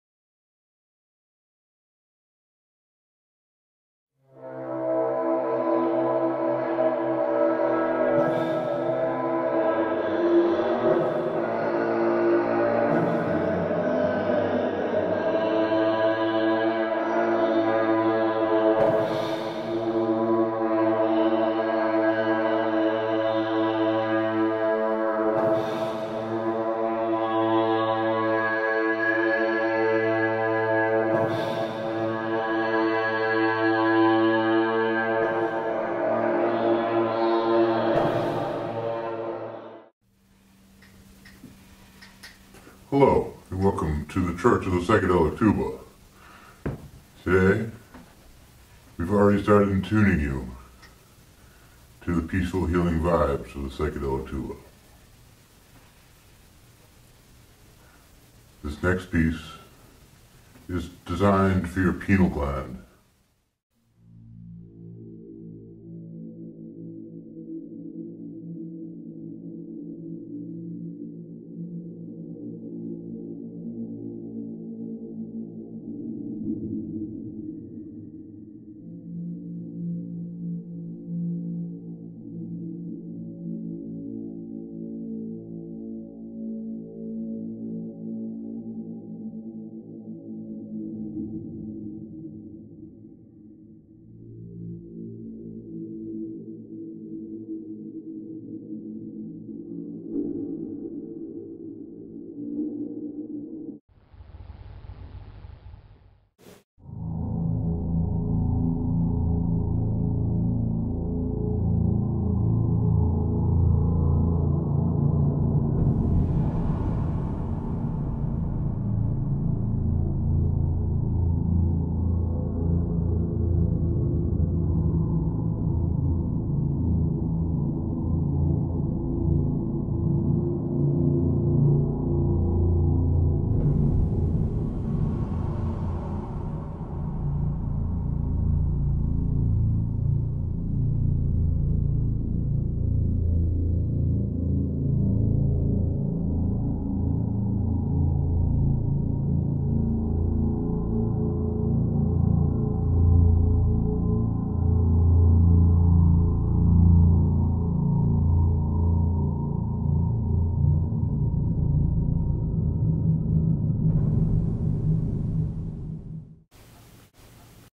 Church of the Psychedelic Tuba Pineal Gland Stimulation
Learn to stimulate your pineal gland with the healing vibes of the psychedelic tuba.